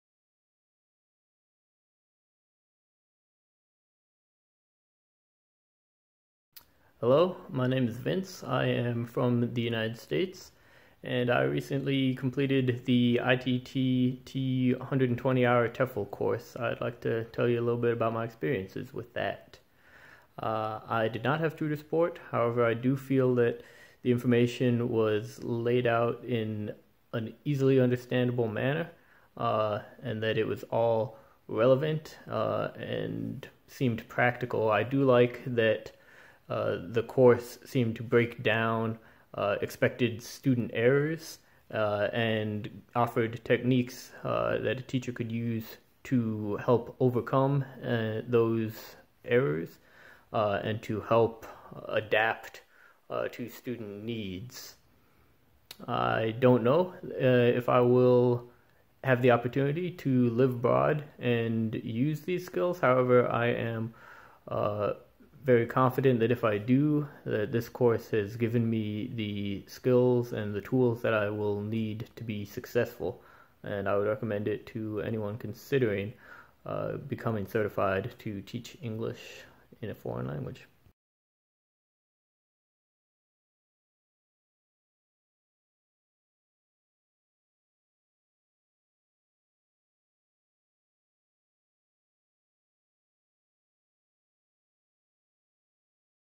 TESOL TEFL Reviews - Video Testimonial – Vince
In this TEFL review video Vince tells us that he graduated from ITTT’s 120-hour online TEFL course. He thought the course was very well structured and liked that it offered techniques to overcome the most common teaching errors. He hopes to travel and teach English abroad in the near future.